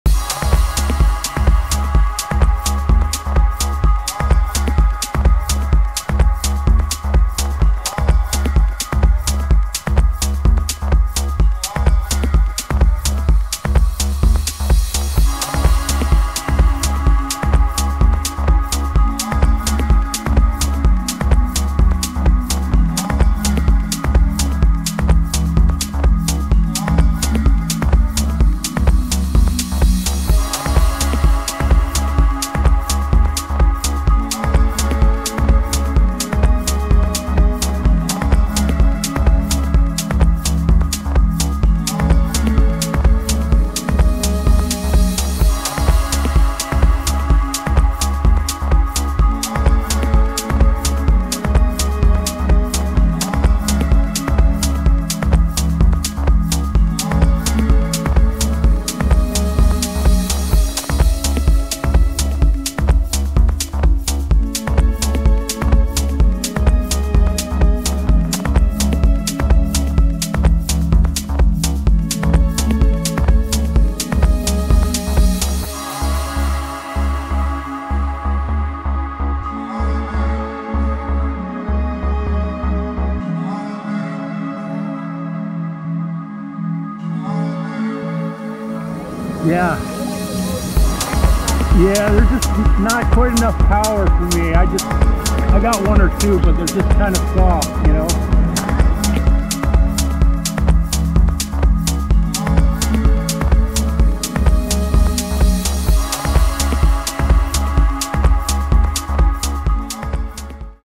Winter morning surf
Unusually chilly morning in the mid 40's...water temp 60